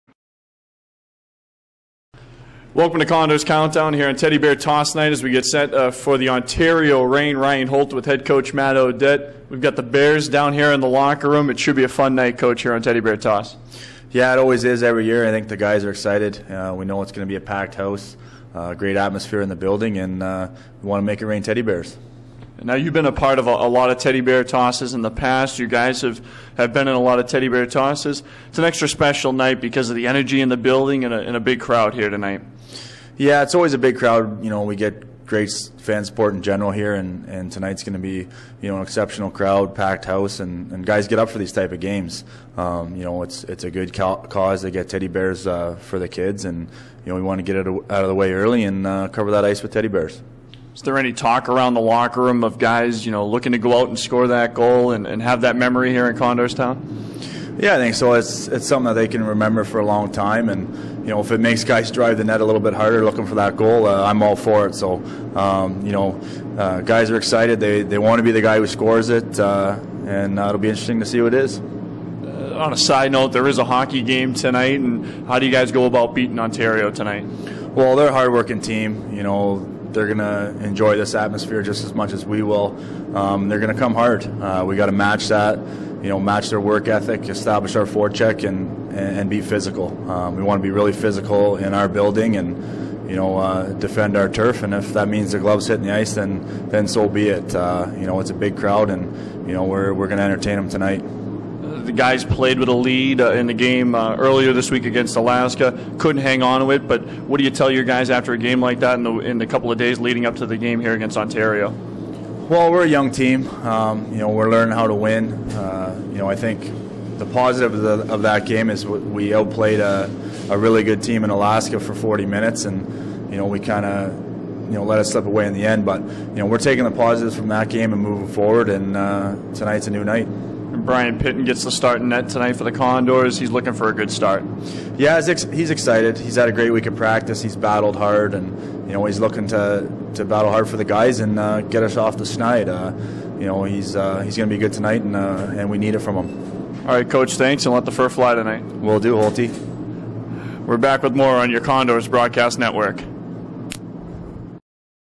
Teddy Bear Toss Condors Countdown
Ryan Holt and Matt O'Dette are joined by a fuzzy friend before tonight's Teddy Bear Toss game on 11/26 in Condorstown